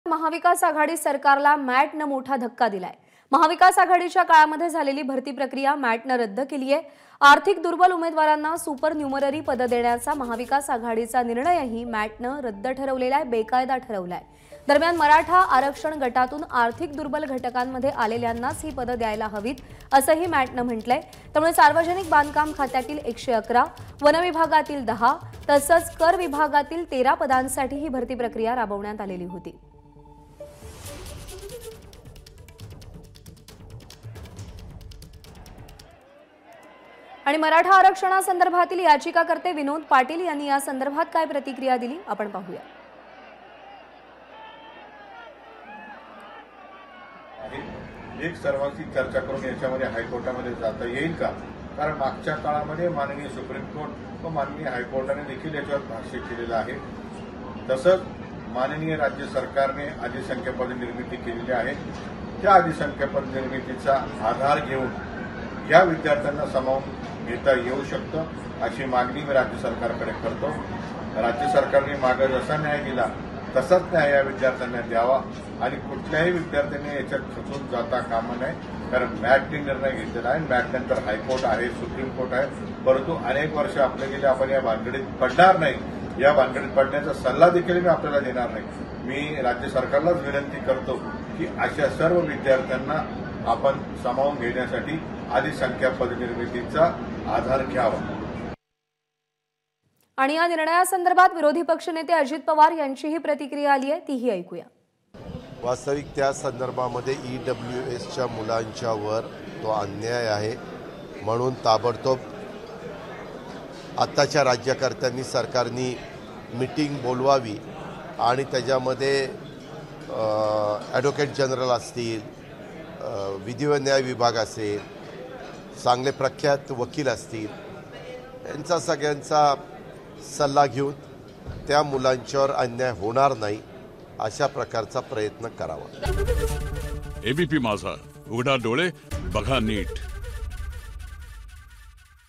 EWS Maharashtra : महाविकास आघाडी सरकारला मॅटचा धक्का, EWSबाबतच्या 'त्या' निर्णयात बदल ABP Majha
महाविकास आघाडी सरकारला मॅटचा धक्का,  आर्थिक दुर्बल उमेदवारांना सुपरन्युमररी पदे देण्याचा महाविकास आघाडीचा निर्णय मॅटने बेकायदा ठरवला, तर मराठा आरक्षण गटातून आर्थिक दुर्बल घटकांत आलेल्यांनाच ही पदे देण्याचे निर्देश.